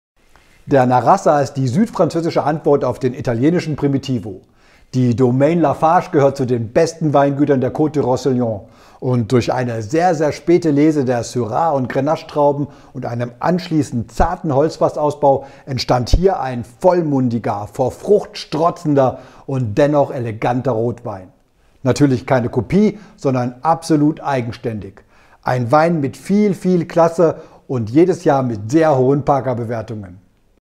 Domaine Lafage la Narassa / Wein Frankreich
Die Cuvée Narassa wurde aus Grenache und Syrah erzeugt und im Holzfass ausgebaut.